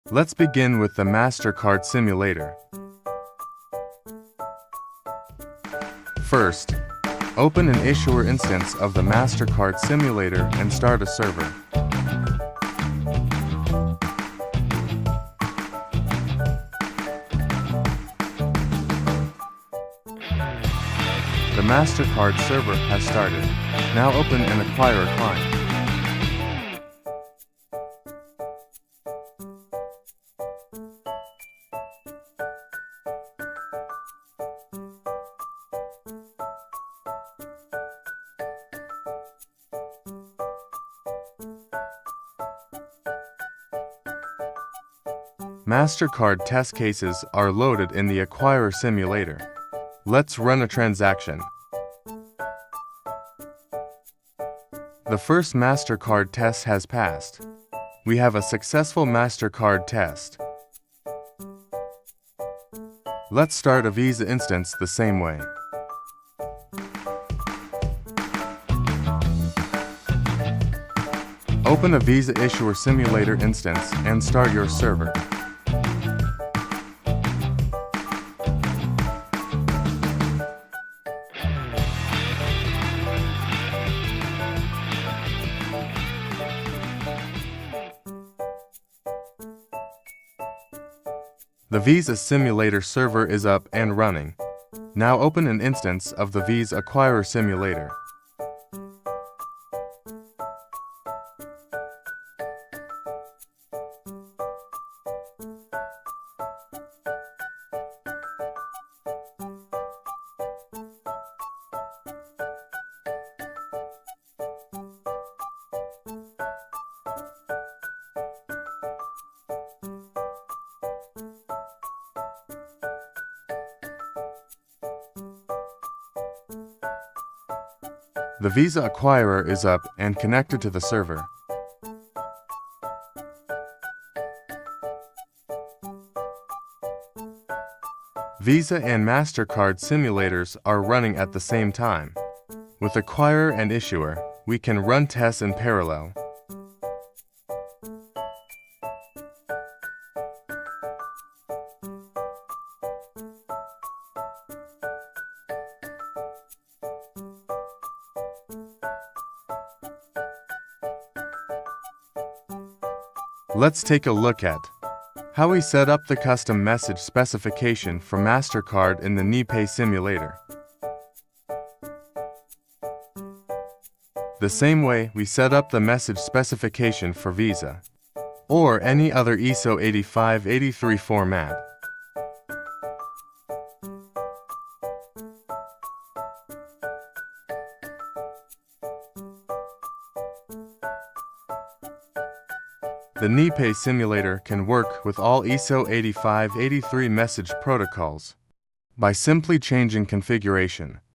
neaPay ISO858 simulator demo setup for VISA and MasterCard
The neaPay ISO858 simulator can be setup for any format, including VISA and MasterCard , online and clearing. One simulator for all connections.